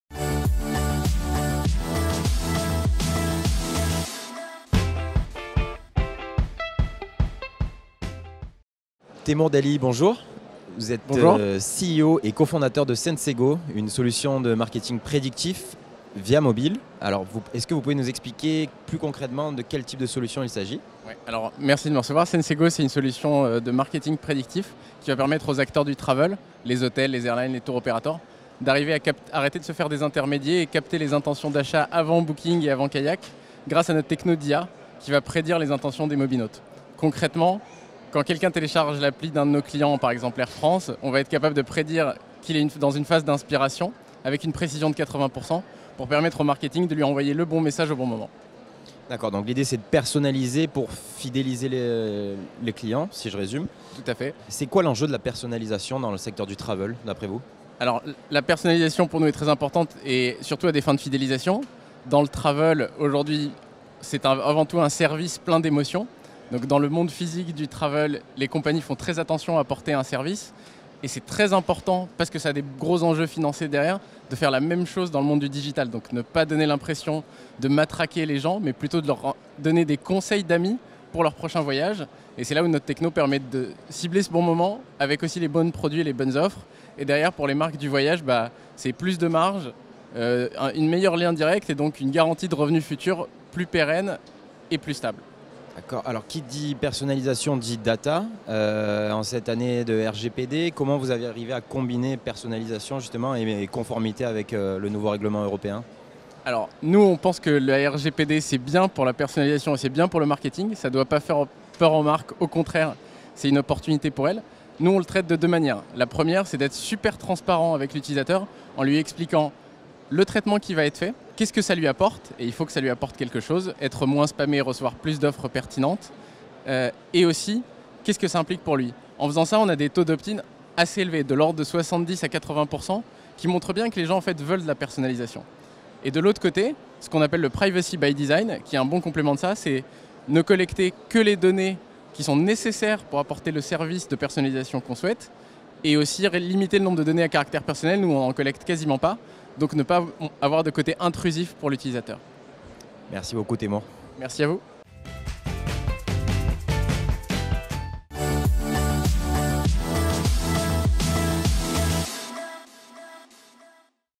IFTM2018 : Sensego, une solution de marketing prédictif dédiée aux parcours sur mobile
Sur le studio de l'IFTM Top Resa 2018, TOM a accueilli Teimour Daly, Co-fondateur et CEO de Sensego. L'occasion de comprendre comment fonctionne la solution de marketing prédictif développée par la startup, mais aussi de revenir sur les enjeux de la personnalisation dans le Travel.